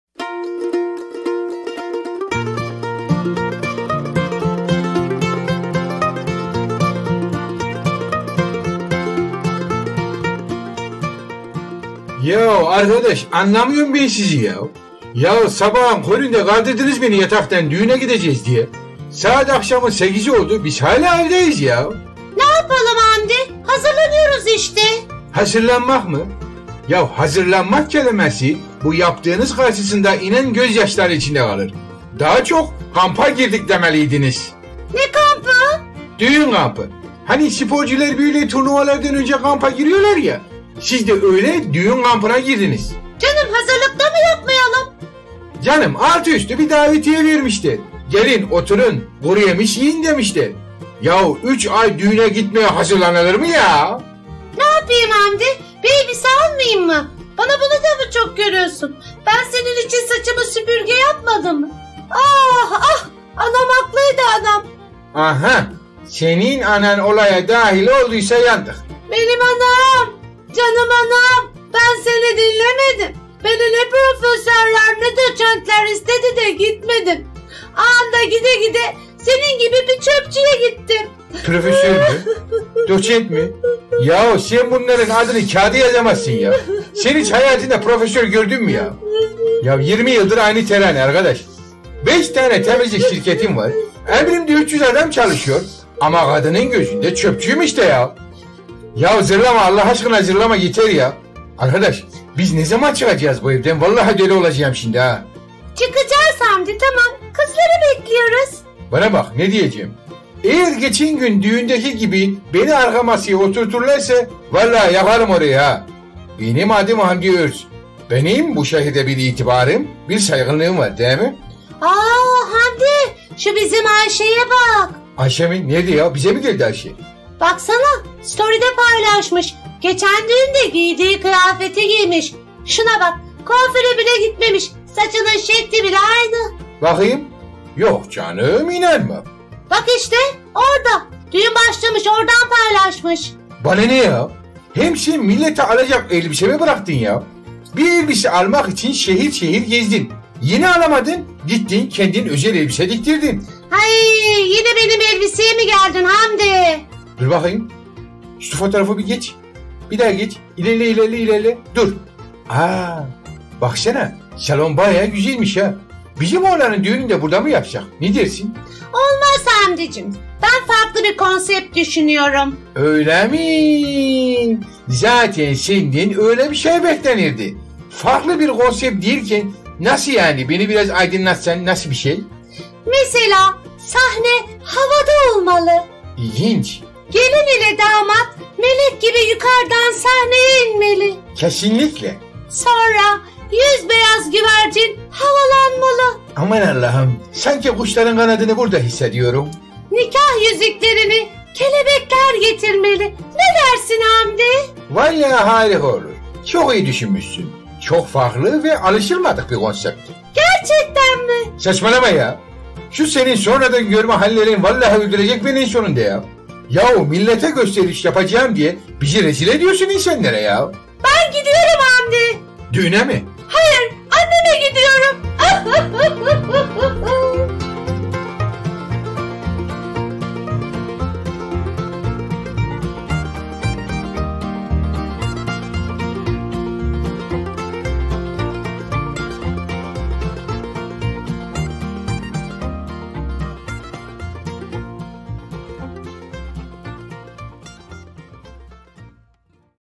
Dügüne Gidiyoruz
Hamdi Dügüne gitmek icin hazir. Pakize hem hazirlaniyor hem de baska bir dügünün planlarini yapiyor